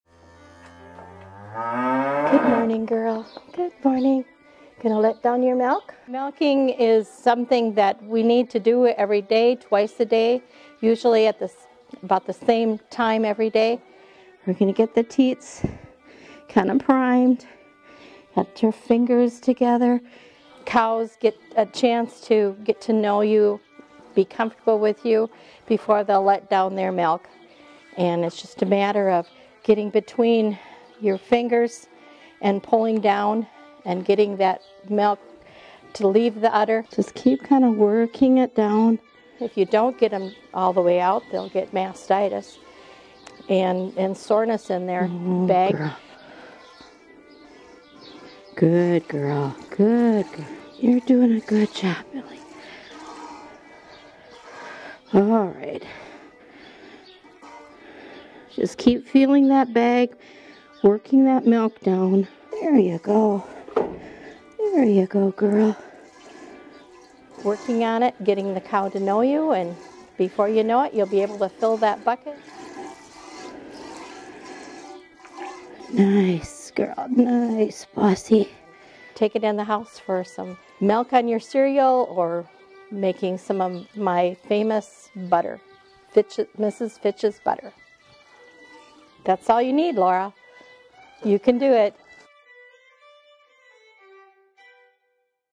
What Does a Cow Say?
Laura learned to be self reliant.  Mrs. Nelson taught her how to milk.  She was busy to night so we asked Mrs. Fitch to explain how to milk a cow. We bring a few of the old pioneer skills to you to show how they were done. This recording first aired on July 11 ,2020.